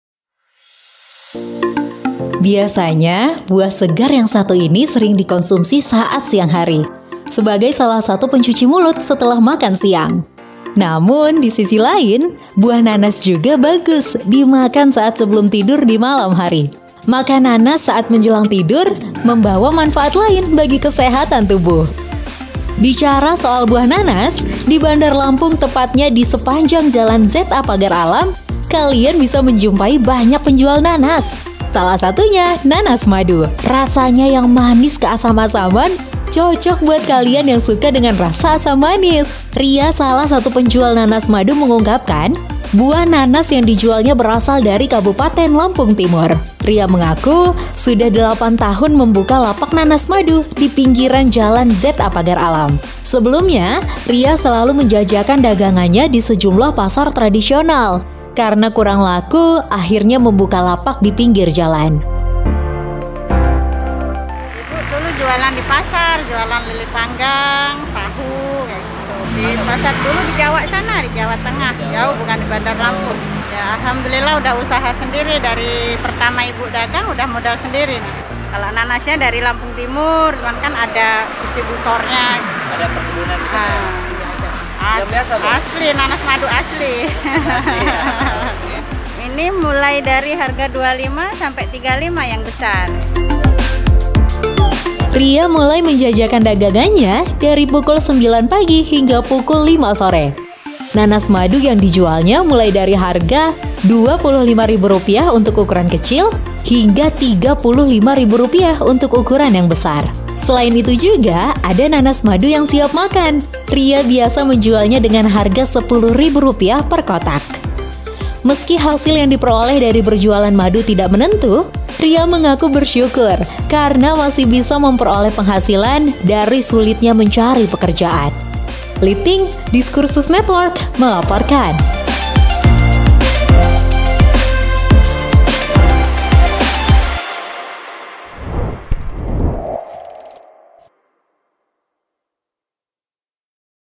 Berburu Nanas Madu di Sepanjang Jalan ZA Pagar Alam Bandar Lampung | Diskursus Network
Berburu Nanas Madu di Sepanjang Jalan ZA Pagar Alam Bandar Lampung
Biasanya, buah segar yang satu ini sering dikonsumsi saat siang hari sebagai salah satu pencuci mulut setelah makan siang. Namun di sisi lain, buah nanas juga bagus dimakan saat sebelum tidur di malam hari. Makan nanas saat menjelang tidur membawa manfaat lain bagi kesehatan tubuh.

Bicara soal buah nanas, di Bandar Lampung tepatnya di sepanjang Jalan ZA Pagar Alam, kalian bisa menjumpai banyak penjual nanas, salah satunya nanas madu. Rasanya yang manis ke asam-asaman cocok buat kalian yang suka dengan rasa asam manis.

Ria salah satu penjual nanas madu mengungkapkan, buah nanas yang dijualnya berasal dari Kabupaten Lampung Timur. Ria mengaku sudah delapan tahun membuka lapak nanas madu di pinggiran Jalan ZA Pagar Alam.

Sebelumnya, Ria selalu menjajakan dagangannya di sejumlah pasar tradisional, karena kurang laku akhirnya membuka lapak di pinggir jalan.

Ria mulai menjajakan dagangannya dari pukul 09.00 WIB hingga pukul 17.00 WIB. Nanas madu yang dijualnya mulai dari Rp25.000 untuk yang ukuran kecil hingga Rp35.000 untuk ukuran yang besar.Selain itu juga ada nanas madu yang siap makan, Ria biasa menjualnya dengan harga Rp10.000 per kotak.

Meski hasil yang diperoleh dari berjualan madu tidak menentu, Ria mengaku bersyukur karena masih bisa memperoleh penghasilan dari sulitnya mencari kerja.

Kami juga hadir di:
Play Store :